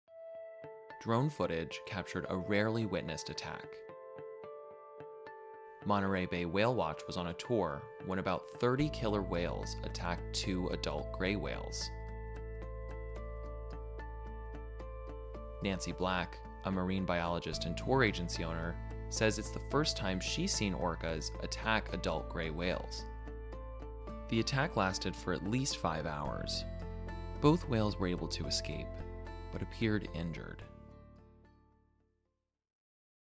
Drone footage captures rare whale attack
About 30 orcas attacked two gray adult whales in California. Both whales were injured but able to escape.